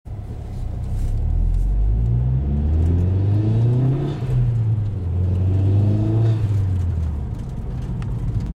CorkSport 2010 Mazdaspeed3 Intake, Racepipe, Downpipe First Gear Pull
Quick clip of the CorkSport 2010 Mazdaspeed3 with CorkSport Power Series Intake & Turbo Inlet Pipe, Racepipe and Downpipe.  Intake/Turbo Inlet ($239) & Racepipe (from $149) available here:

Downpipe soon to be released.